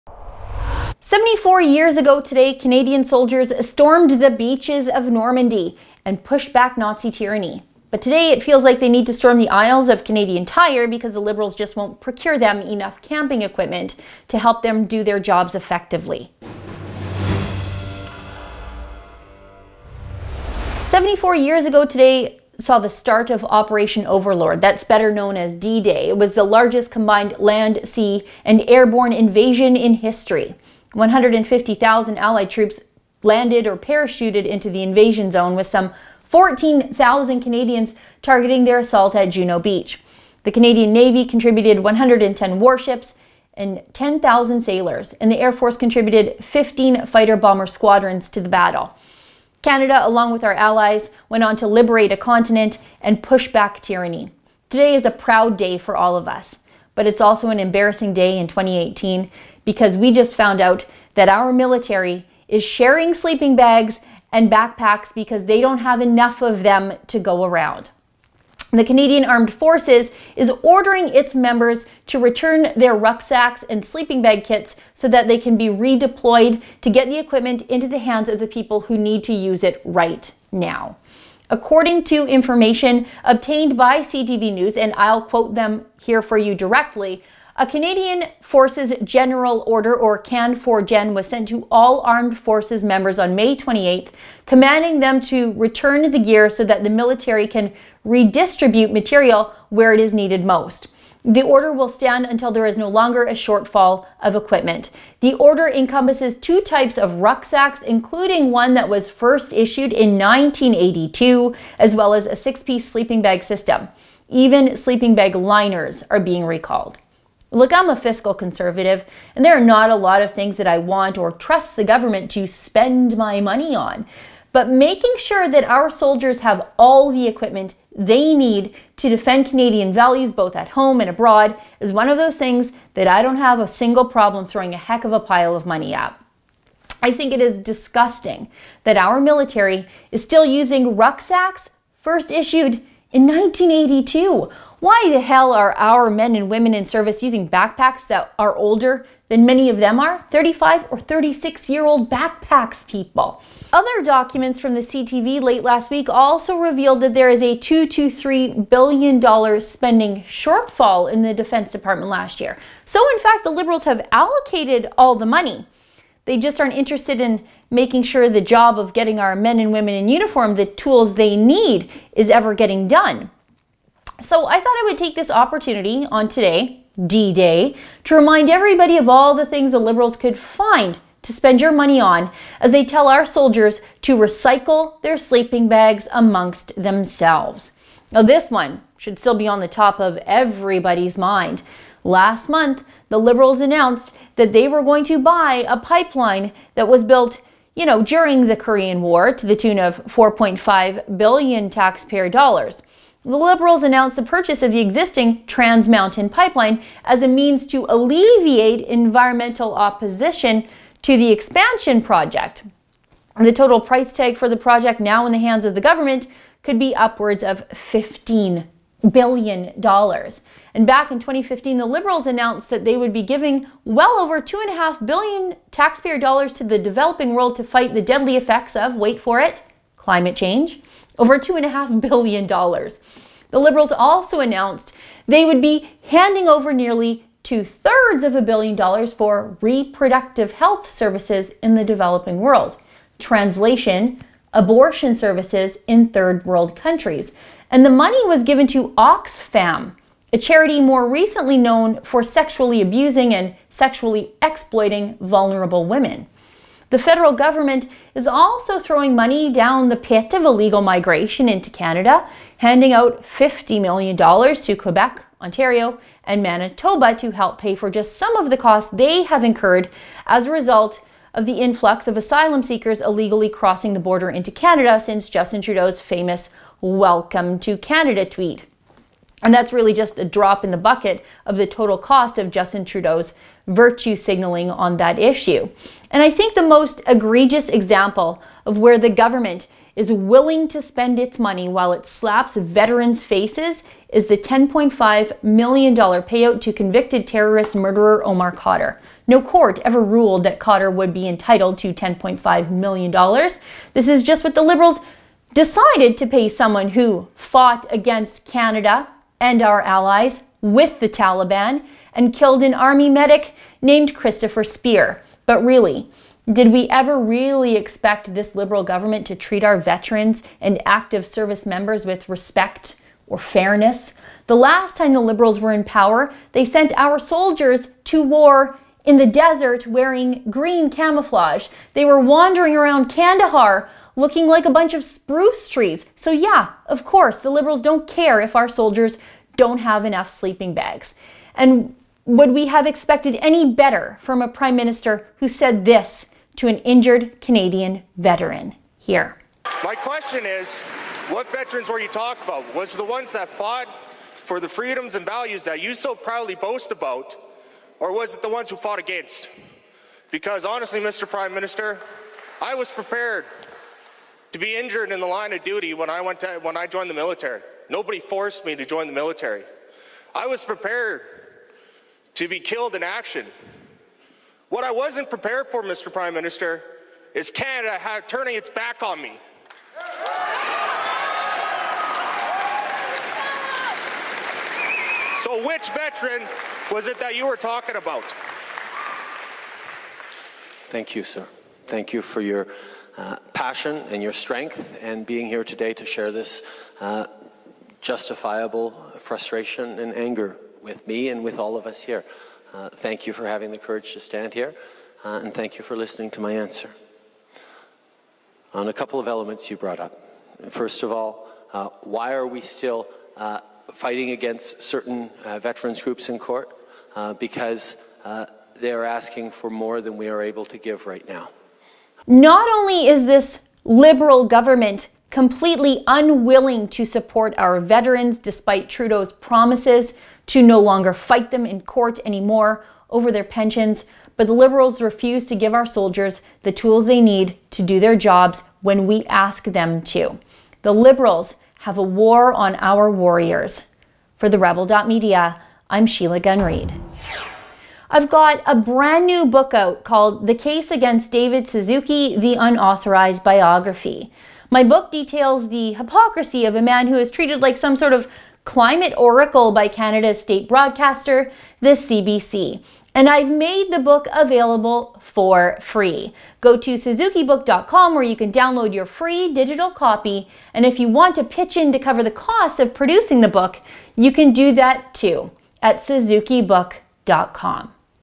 Trudeau Liberals cause military equipment “shortfall” | Sheila Gunn Reid
Sheila Gunn Reid of The Rebel.Media reports: Seventy-four years ago, Canadian soldiers stormed the beaches of Normandy and pushed back tyranny but today, they need to storm the aisles of Canadian Tire because the Liberal government won't procure enough camping equipment for them. MORE
SIGN UP to see Sheila Gunn Reid’s Gunn Show (PLUS shows hosted by Ezra Levant, John Cardillo and MORE)